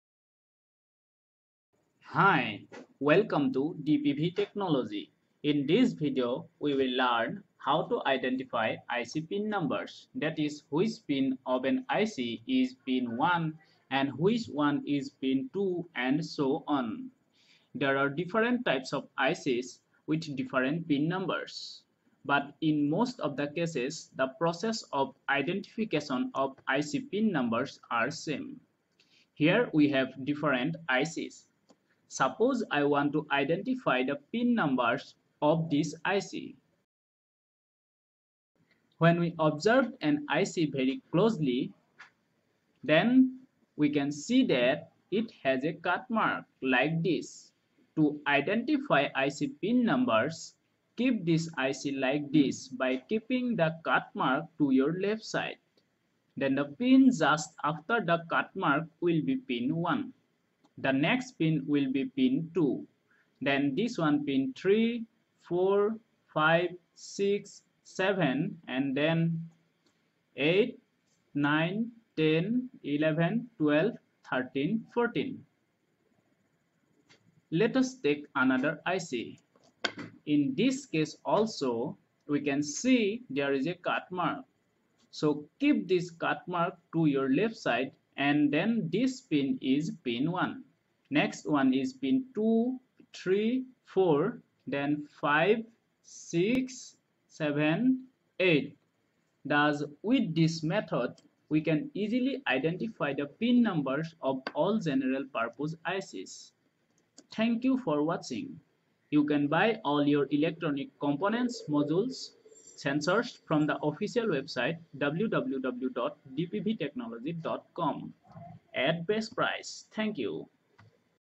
How to read / identify / check Ic pin number [ CC ]
This video will show you how to check / identify / know integrated circuit ( IC ) pin number.